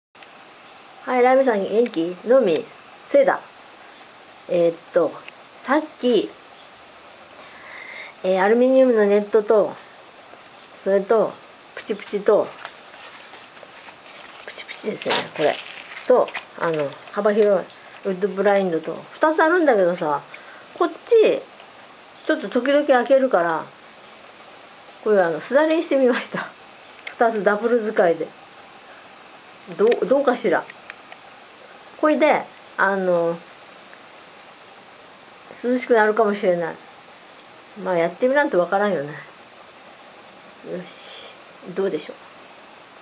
エコだ。ウッドブラインドとプチプチとアルミのネットとすだれ（*^_^*）
右側は、開けないが、左側は、雨戸の開け閉めに使う。茶室の躙り口と思えば、下半分だけが、使えればいいかも。